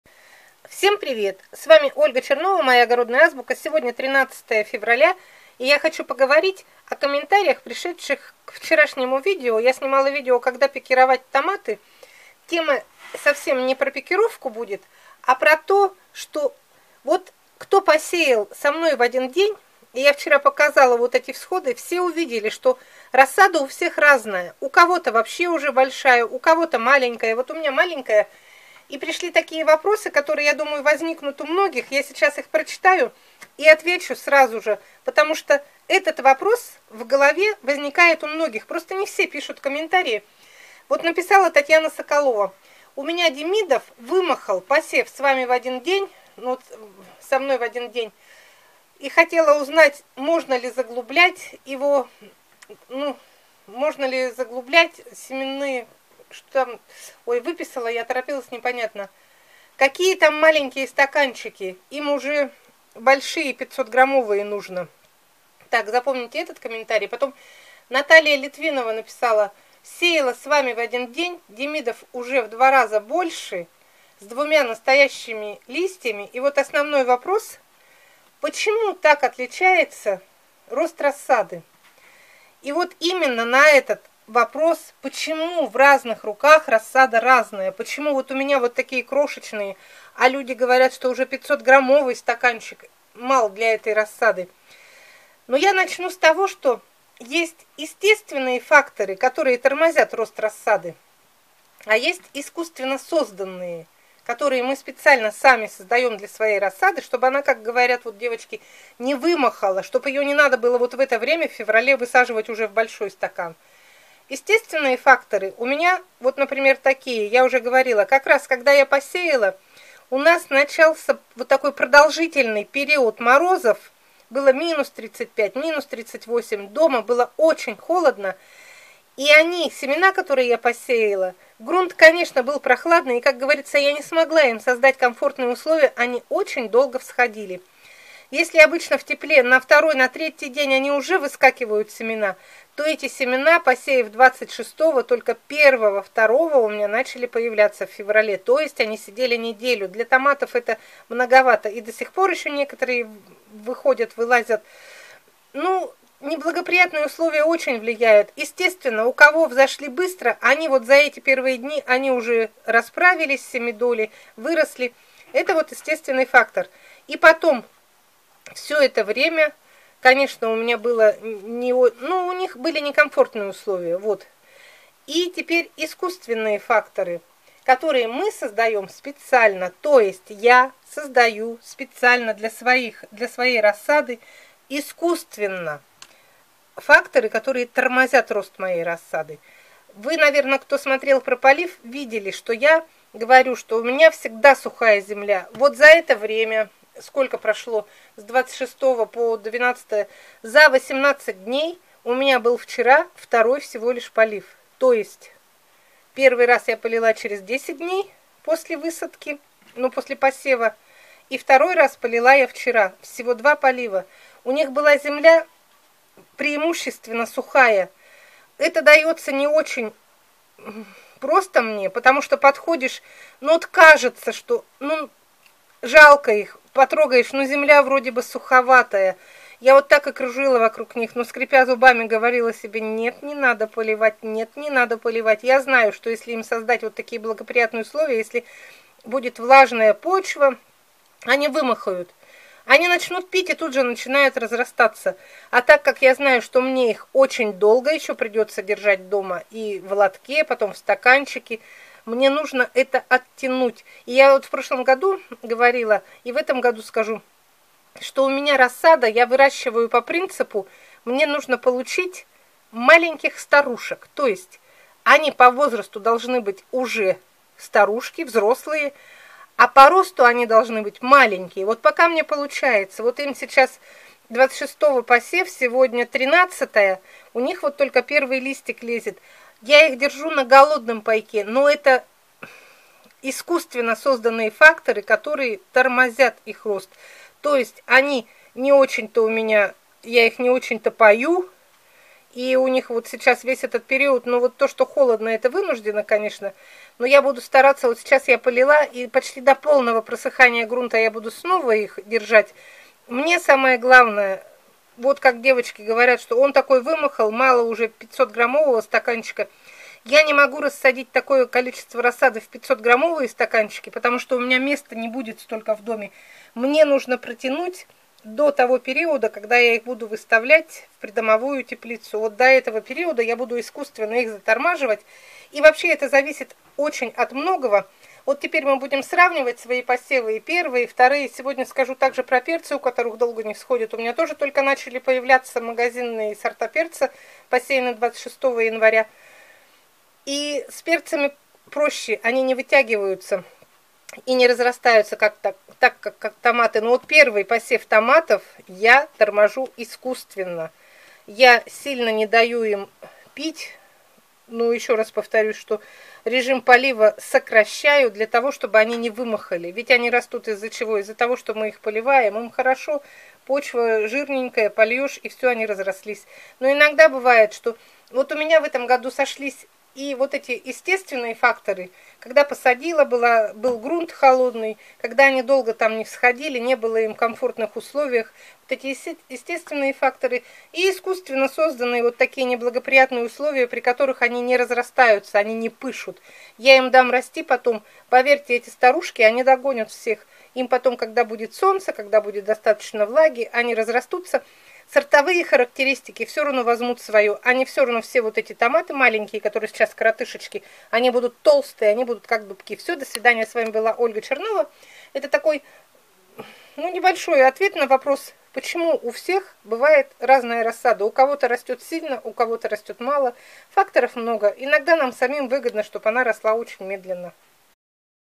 ПОЧЕМУ РАССАДА ТОМАТОВ У ВСЕХ РАЗНАЯ? Ольга Чернова.
Если мы начнем сравнивать свою рассаду томатов, посеянную в один день, мы увидим насколько она разная. У одних - уже огромная, у других - совсем крошечная!
Отчего это зависит? Почему она у всех такая разная?
В этом видео я отвечу на вопросы моих зрителей канала ЮТУБ.